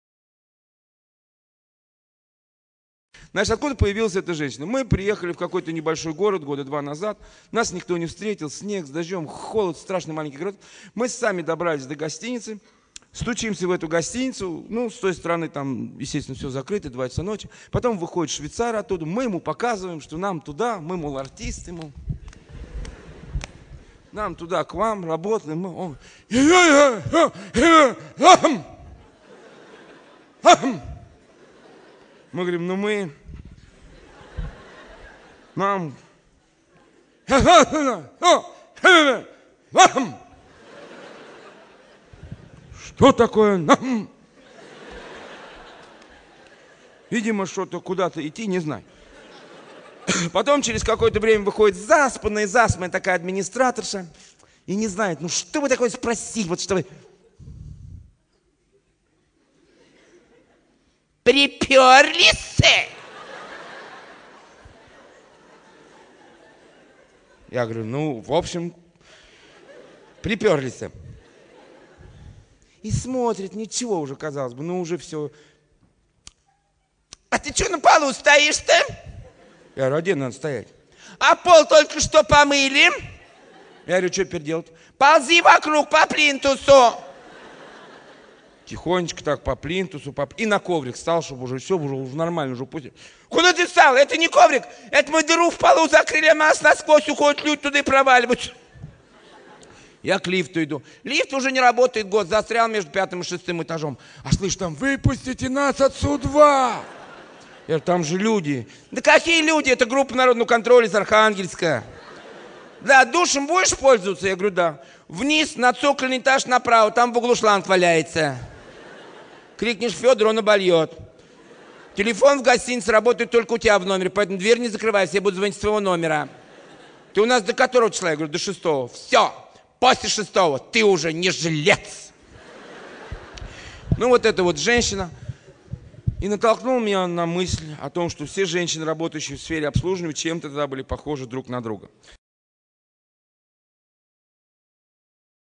Ян АРЛАЗОРОВ ГОСТИНИЦА 100% СМЕХ ДО СЛЕЗ!!
Ян АРЛАЗОРОВ ГОСТИНИЦА

Ян Арлазоров (Yan Arlazorov) выдающийся юморист, который навсегда останется одним из самых ярких представителей в истории своего жанра. Он умел смешить и умел смеяться. Выступления Арлазорова основывались на тесном контакте с публикой.  Стараясь совершить больше, Ян постоянно гастролировал, придумывал новые сценки и представления

100% СМЕХ ДО СЛЕЗ!!